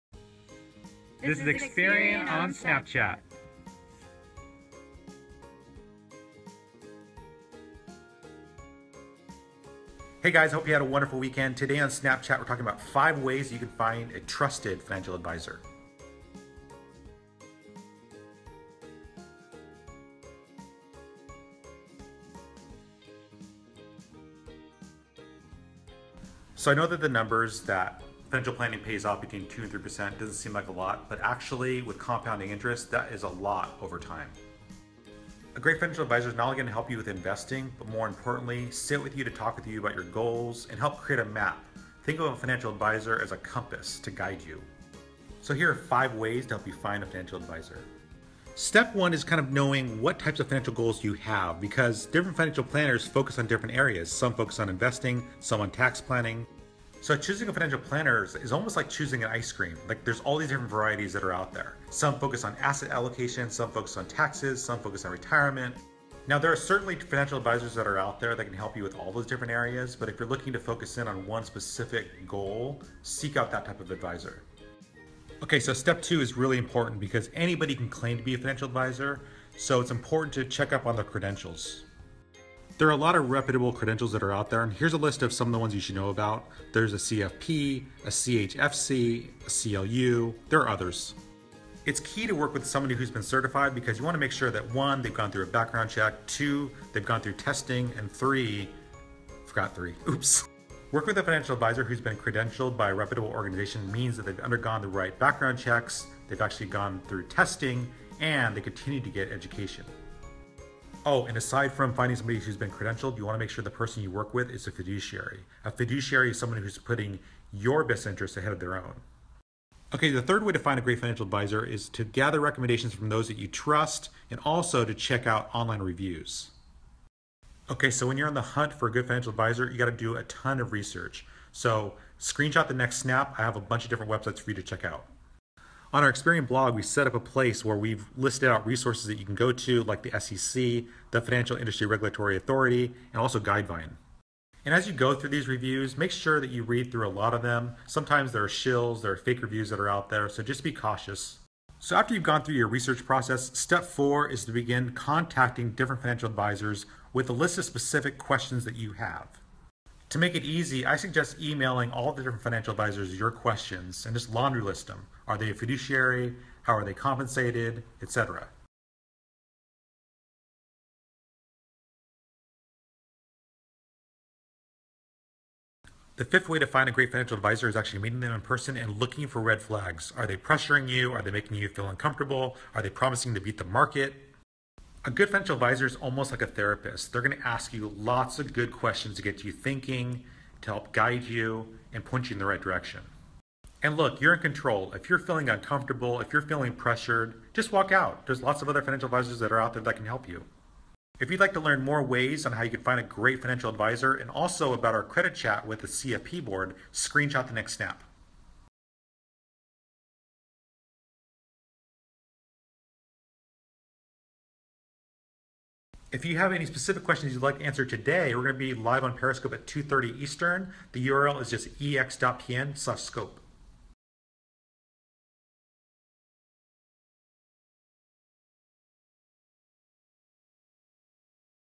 Experian on Snapchat: How to Find a Financial Advisor You Can Trust
If you're struggling to create a financial plan -- or not sure where to start -- you should definitely talk with a financial advisor. Today, we share 5 steps to finding a financial advisor you can trust.

Here is a list of questions to ask your potential financial planner